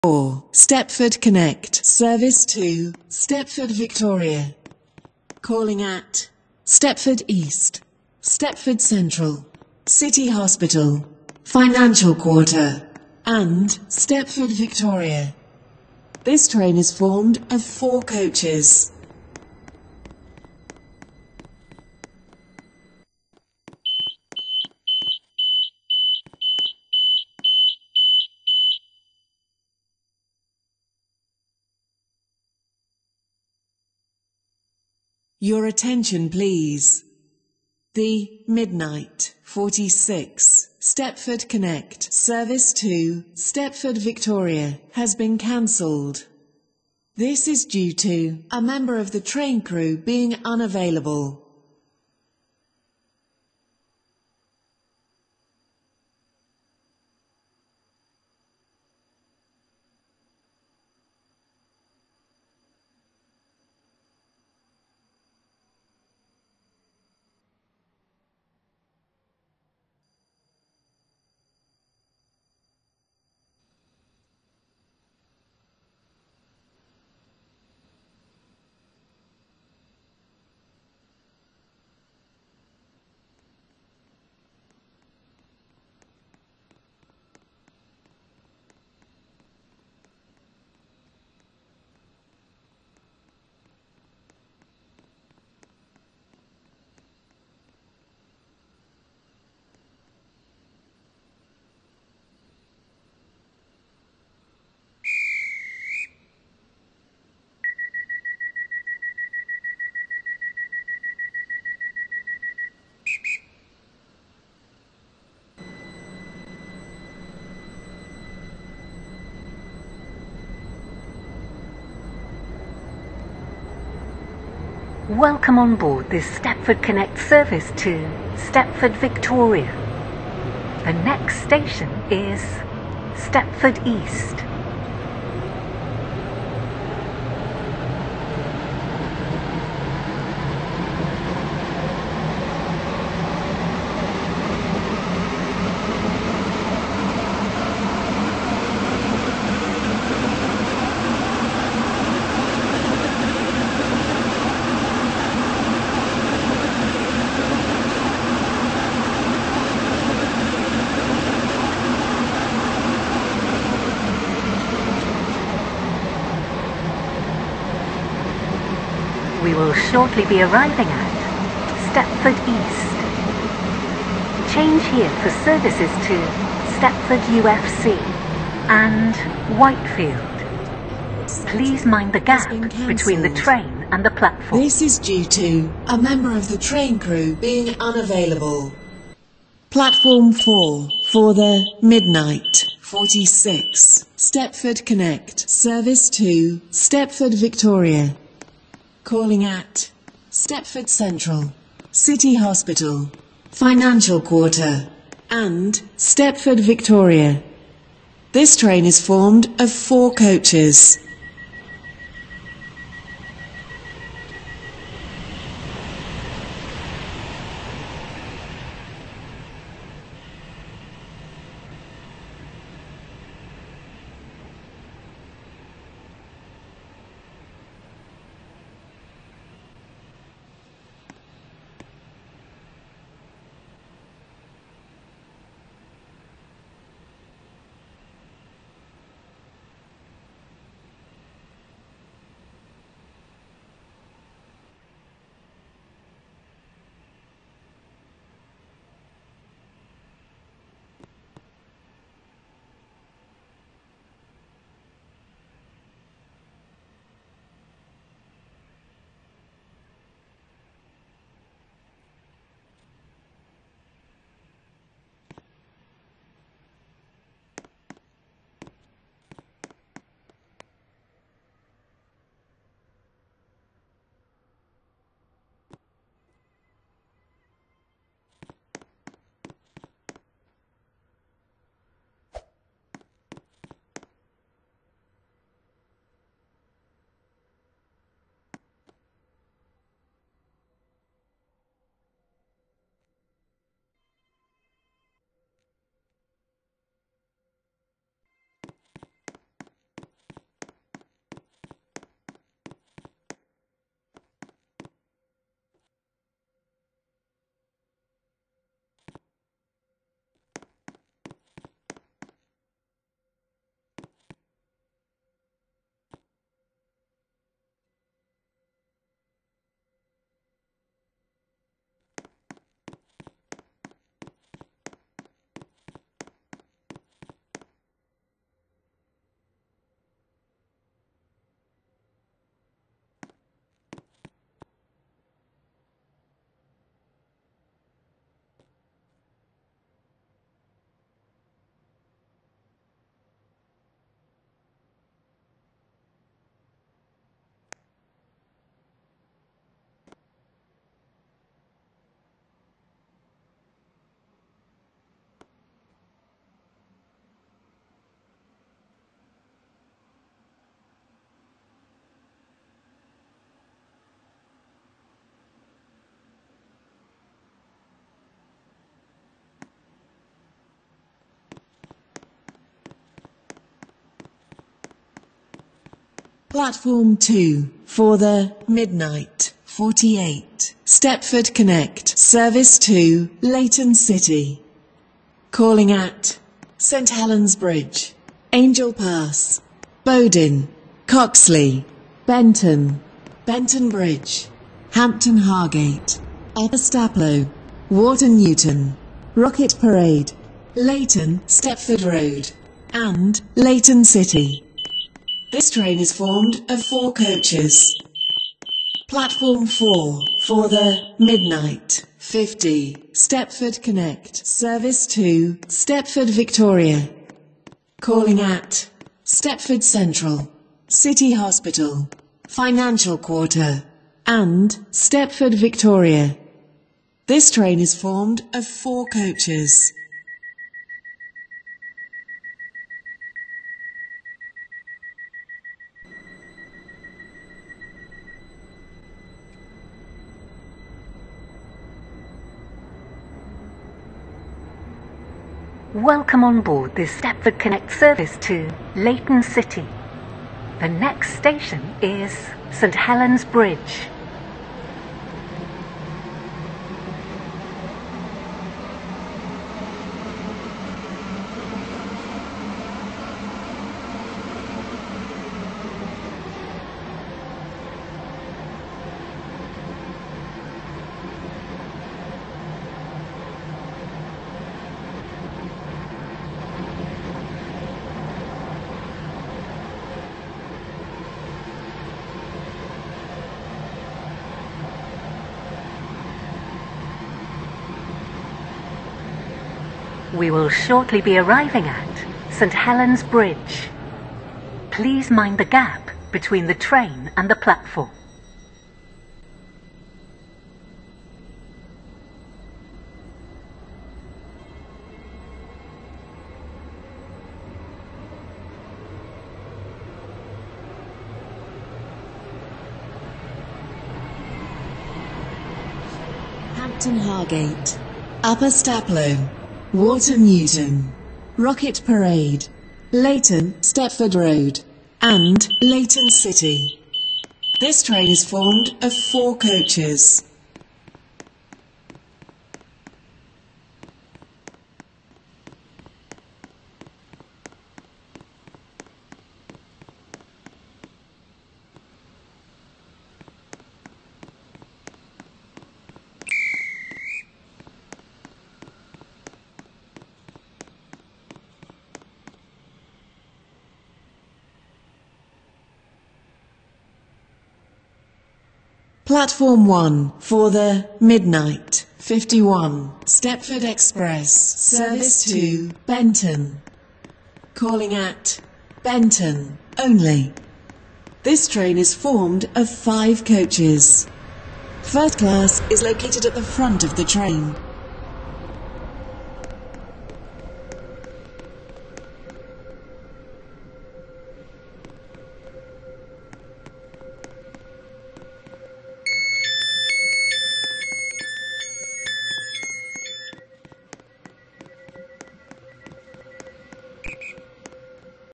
train from st helens bridge to stepford east and back 14/10/23
we went on 2 357s. 357 339 and 357 347